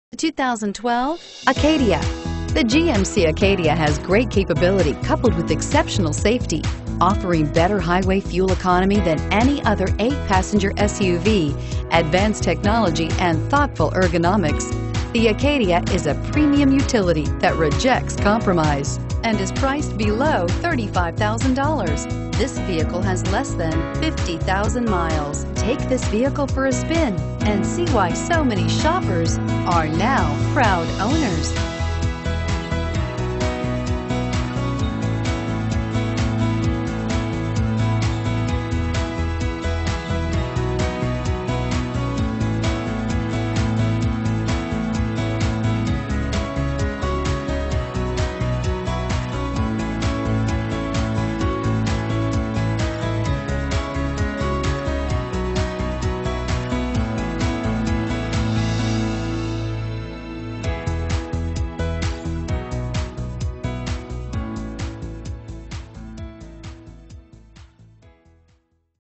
2012 GMC Acadia Grapevine TX GCJ368387
2012 GMC Acadia Denali

Grubbs Infiniti
1500 Highway 114

This smooth-riding 2012 GMC Acadia Denali provides extraordinary options like dual climate control, rear air conditioning, remote starter, anti-lock brakes, parking assistance, traction control, dual airbags, and airbag deactivation. With a safety rating of 5 out of 5 stars, everyone can feel safe. Come by today to see this one in person!